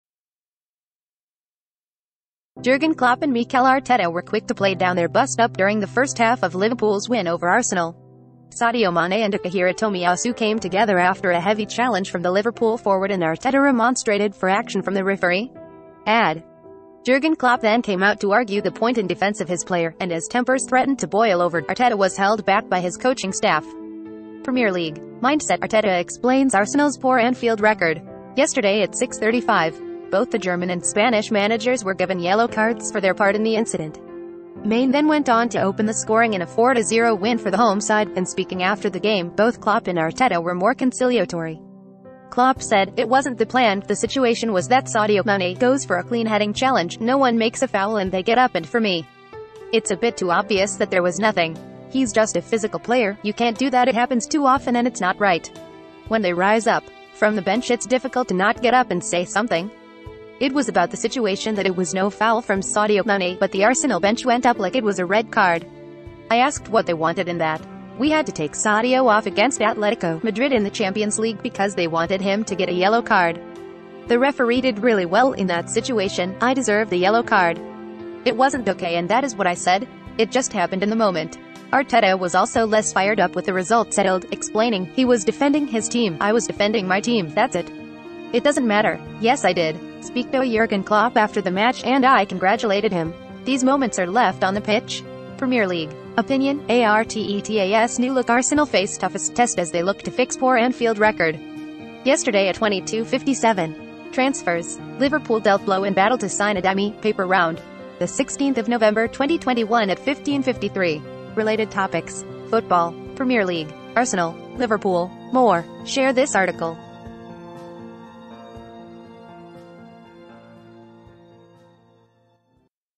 'I deserved a yellow card' - Jurgen Klopp and Mikel Arteta play down bust-up after Liverpool beat A
Jurgen Klopp and Mikel Arteta were quick to play down their bust-up during the first half of Liverpool’s win over Arsenal.Sadio Mane and Takehiro Tomiyasu came together after a heavy challenge from the Liverpool forward and Arteta remonstrated for action from the referee.AdJurgen Klopp then came out to argue the point in defence of his player, and as tempers threatened to boil over, Arteta was held back by his coaching staff.PREMIER LEAGUE'MINDSET' - ARTETA EXPLAINS ARSENAL’S POOR ANFIELD RECOR credit